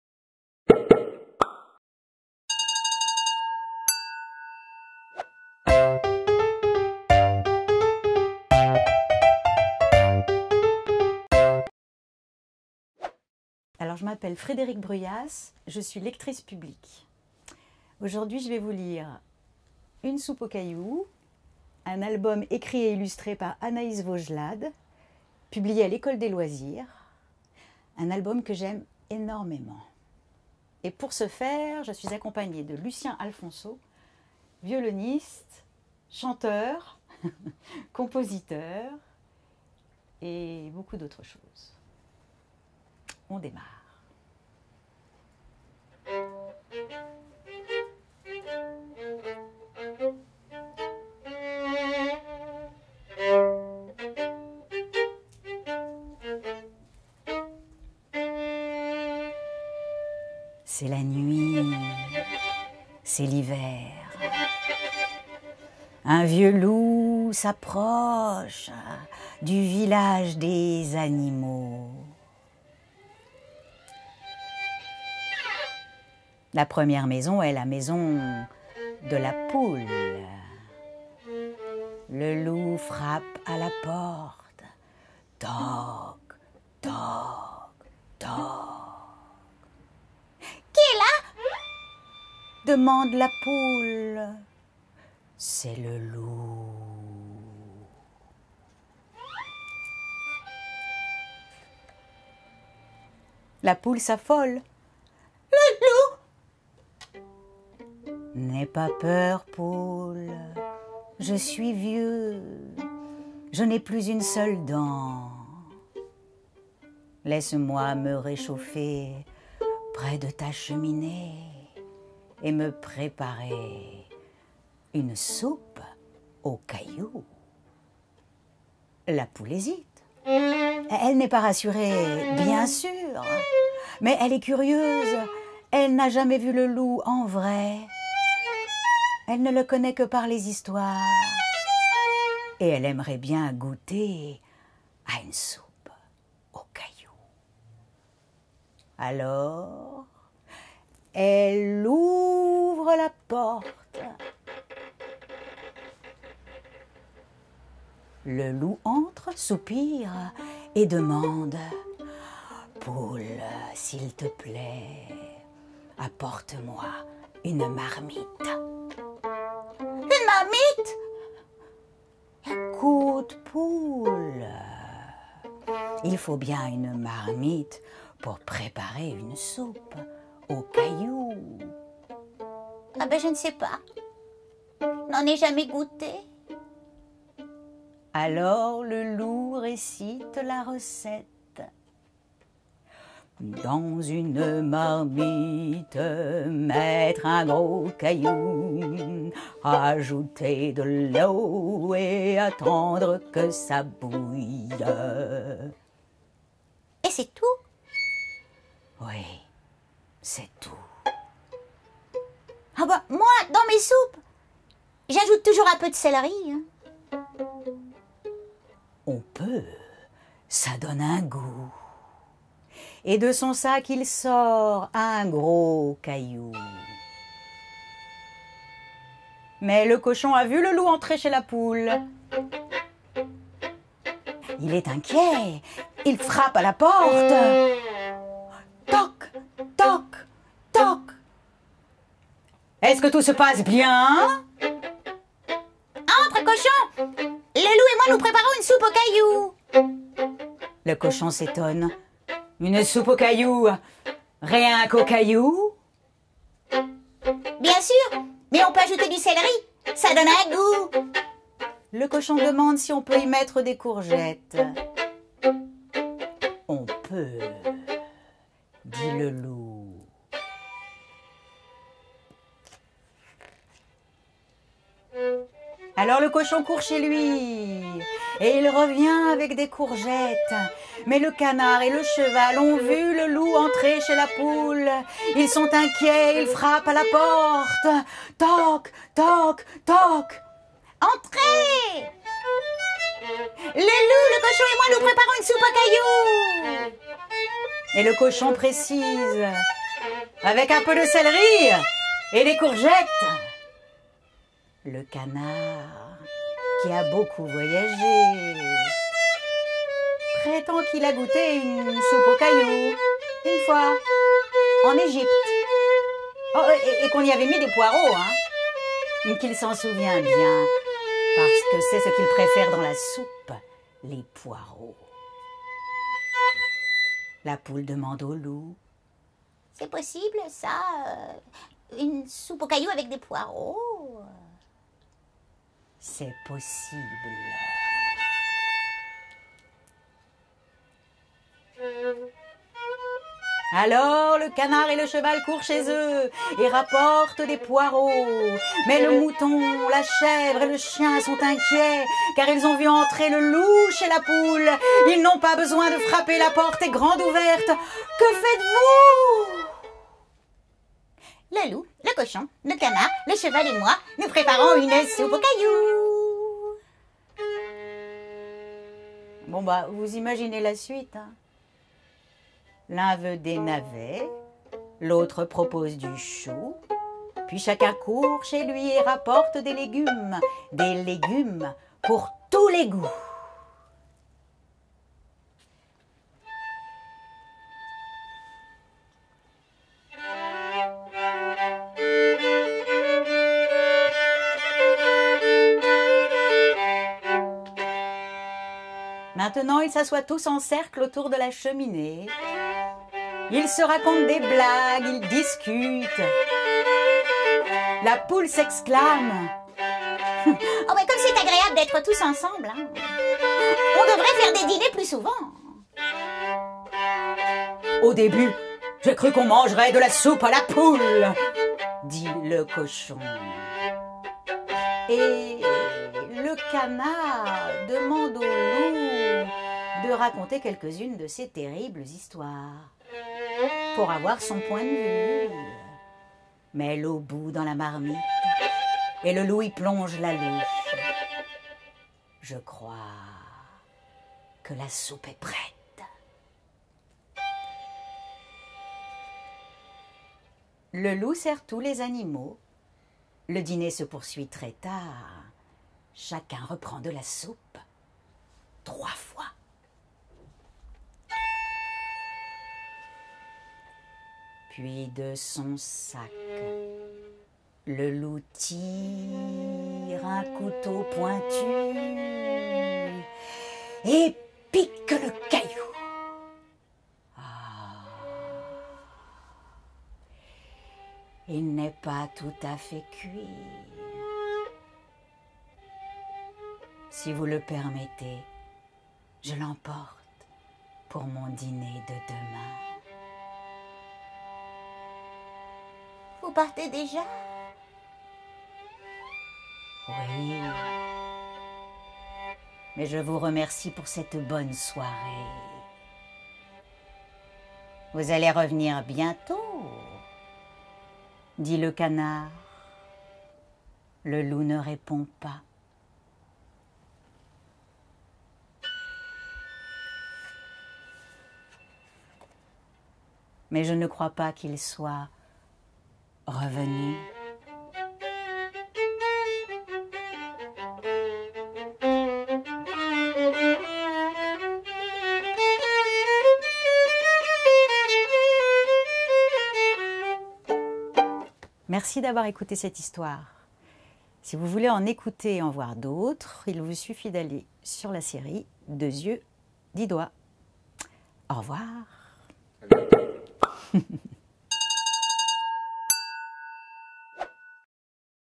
2 yeux 10 doigts : lecture de l'album Une soupe au caillou
2 yeux, 10 doigts, la série vidéo jeunesse des bibliothèques de la Ville de Paris !

Dans cet épisode diffusé à l’occasion de la Nuit de la lecture 2021, Frédérique Bruyas, lectrice publique, nous lit Une soupe au caillou, d'Anaïs Vaugelade (L'école des loisirs).